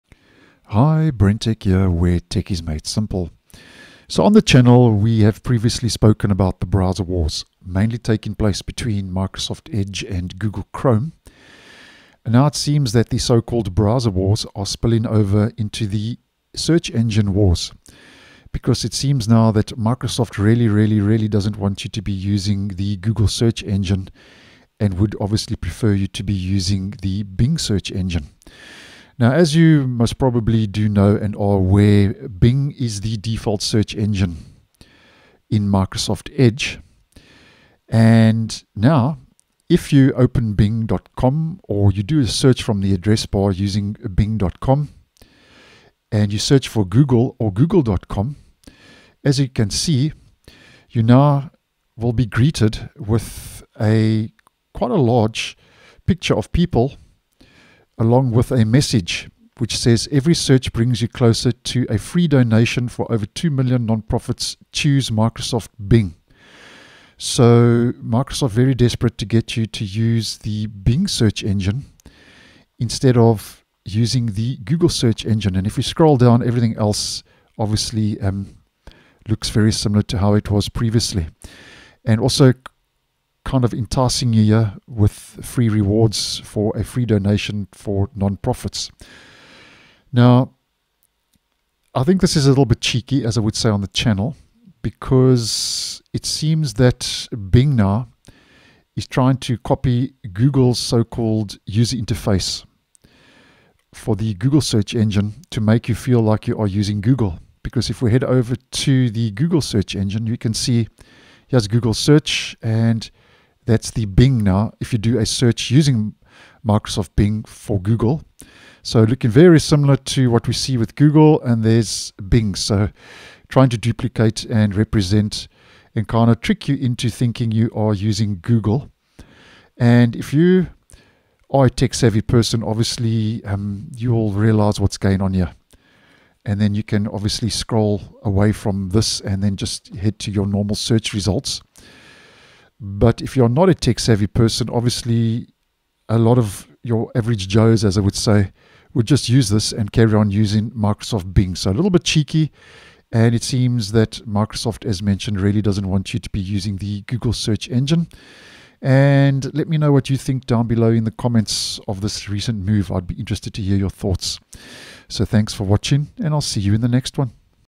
Search Engine Wars: Microsoft REALLY Doesn't Want You Using Google Search!
When using Bing and searching for Google, you’ll now be greeted with a UI looking very similar to Google’s.